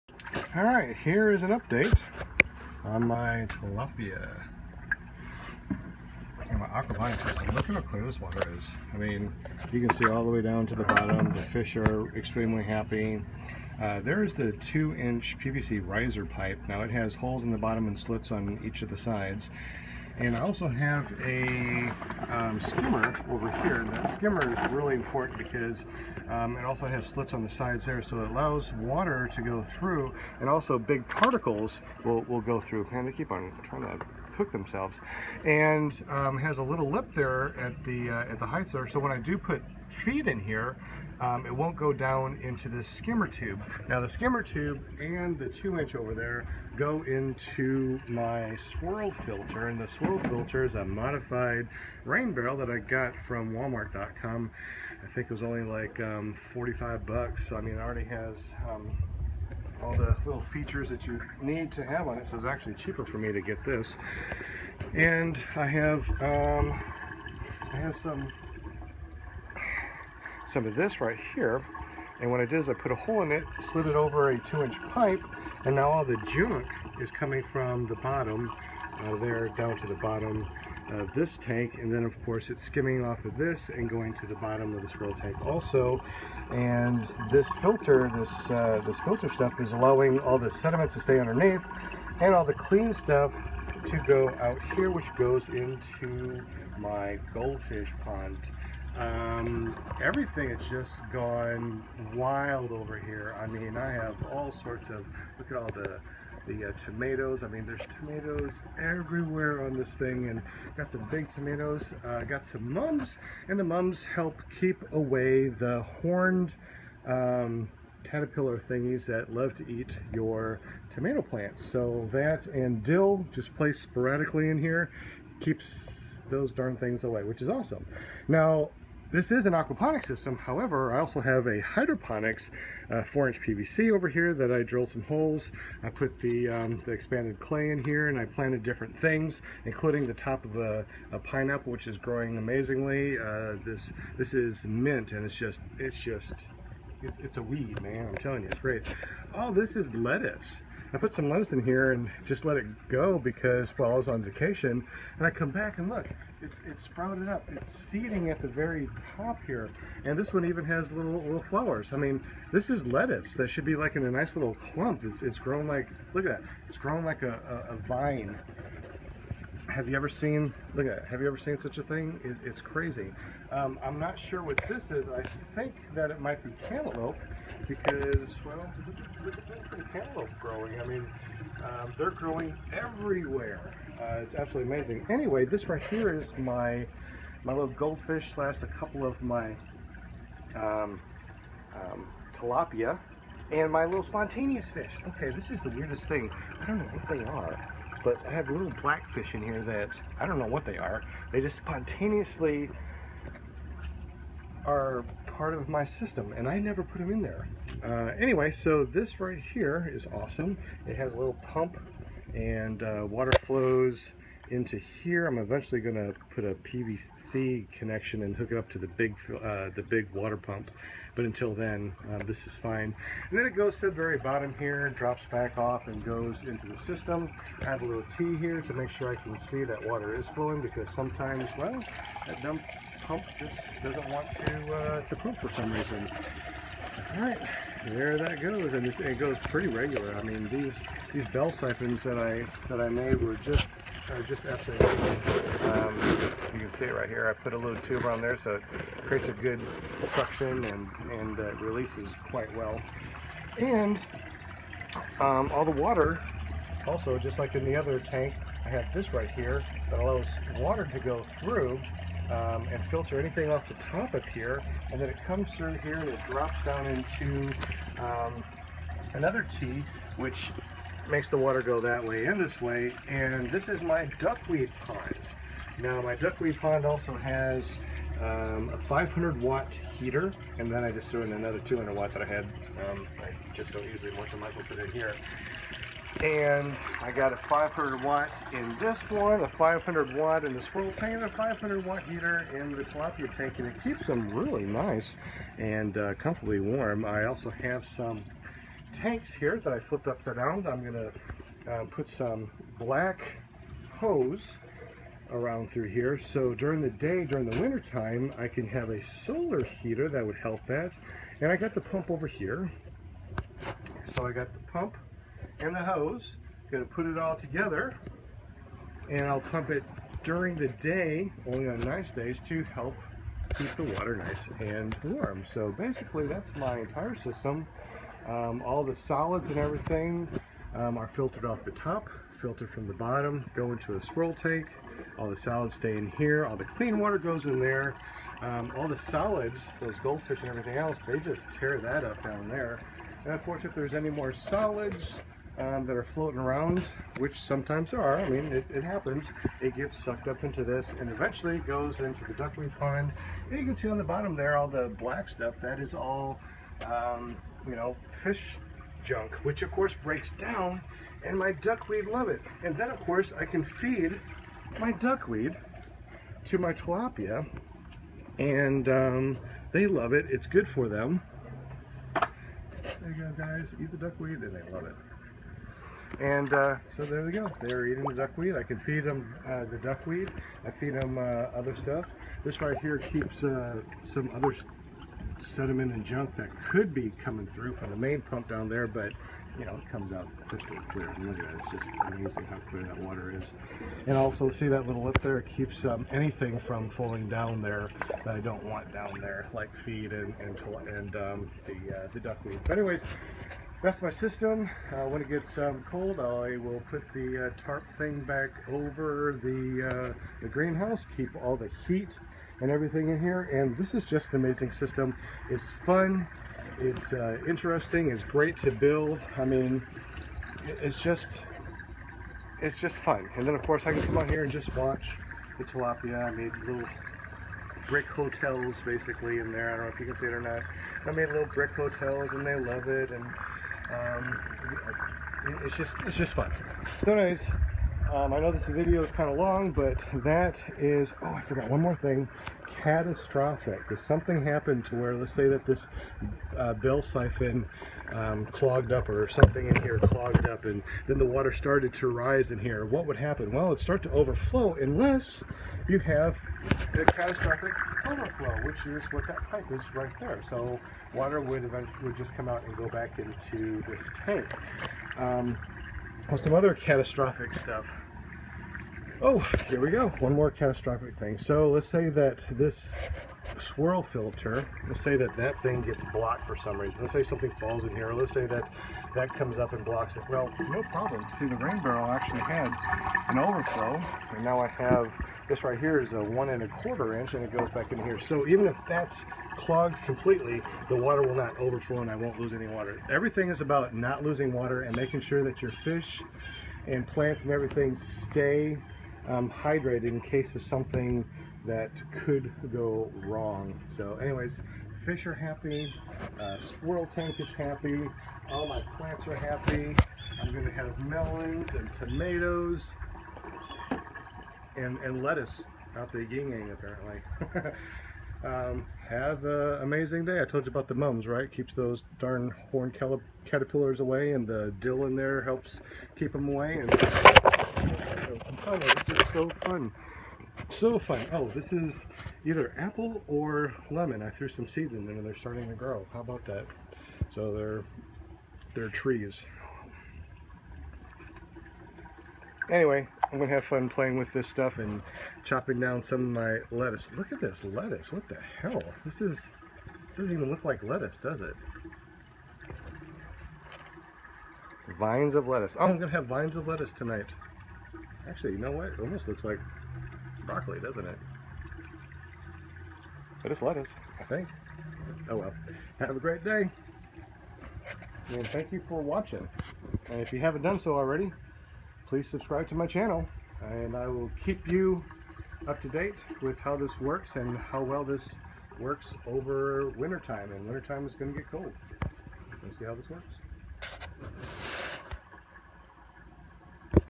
Aquaponics system setup, design concepts and methods
Aquaponics system setup using 2" PVC riser tube from the bottom of the fish tank to the swirl filter which is a modified rain barrel.  Design with IBC containers and expanded clay pellets as grow media.  Swirl filter has two PVC pipes coming in from the fish tank, a 2' from the bottom and an 1" skimmer from the top. Duck weed pond has a small pump that pumps out to the gold fish tank and returns via gravity by a skimmer elbow. tilapia water is clear of algae because of the bacteria in the water system and grown on the walls and bricks in the system.

How to keep the water clear?  Swirl filters, duck weed ponds and letting the system just balance out naturally.  That takes about 30 days...then it is all good

Wiki info about things in this video

Aquaponics /ˈækwəˈpɒnɨks/, refers to any system that combines conventional aquaculture (raising aquatic animals such as snails, fish, crayfish or prawns in tanks) with hydroponics (cultivating plants in water) in a symbiotic environment. In normal aquaculture, excretions from the animals being raised can accumulate in the water, increasing toxicity. In an aquaponic system, water from an aquaculture system is fed to a hydroponic system where the by-products are broken down by nitrification bacteria into nitrates and nitrites, which are utilized by the plants as nutrients, and the water is then recirculated back to the aquaculture system.

As existing hydroponic and aquaculture farming techniques form the basis for all aquaponics systems, the size, complexity, and types of foods grown in an aquaponics system can vary as much as any system found in either distinct farming discipline.[1]

Hydroponics is a subset of hydroculture and is a method of growing plants using mineral nutrient solutions, in water, without soil. Terrestrial plants may be grown with their roots in the mineral nutrient solution only, or in an inert medium, such as perlite or gravel.

Tilapia (/tɨˈlɑːpiə/ ti-LAH-pee-ə) is the common name for nearly a hundred species of cichlid fish from the tilapiine cichlid tribe. Tilapia are mainly freshwater fish inhabiting shallow streams, ponds, rivers and lakes and less commonly found living in brackish water. Historically, they have been of major importance in artisan fishing in Africa and the Levant and are of increasing importance in aquaculture and aquaponics. Tilapia can become problematic invasive species in new warm-water habitats such as Australia,[2] whether deliberately or accidentally introduced, but generally not in temperate climates due to their inability to survive in waters cooler than about 21 °C (70 °F).

Duckweeds, or water lens, are flowering aquatic plants which float on or just beneath the surface of still or slow-moving bodies of fresh water and wetlands. Also known as "bayroot," they arose from within the arum or aroid family (Araceae),[1] and therefore, often are classified as the subfamily Lemnoideae within the Araceae. Classifications created prior to the end of the 20th century classify them as a separate family, Lemnaceae.